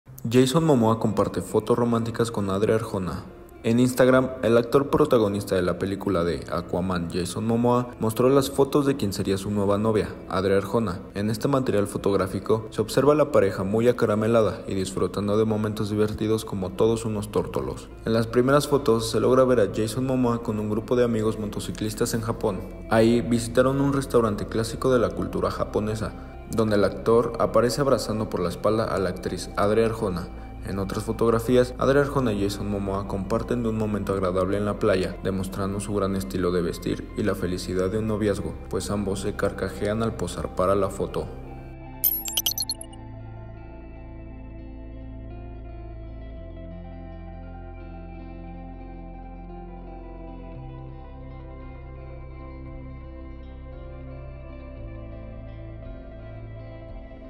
¿Son novios? Jason Momoa comparte fotos románticas con Adria Arjona
El actor protagonista de las películas de ‘Aquaman’, Jason Momoa, mostró las fotos junto con quien sería su nueva novia, Adria Arjona